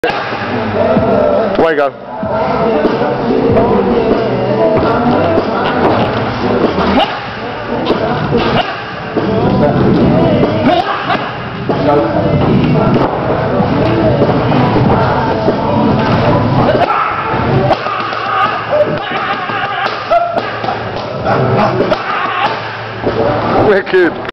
Saint George Groves doing pads with Adam Booth
Saint George Groves doing pads with Adam Booth, before his Oct 4th bout at york hall in the Hayemaker gym, London.